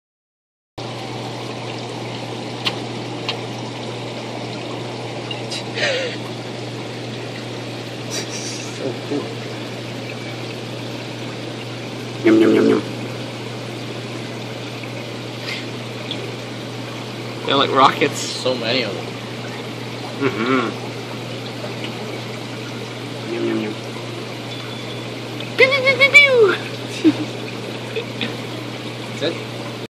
Barnacle Blennies
So cute...like little rockets when they are being fed.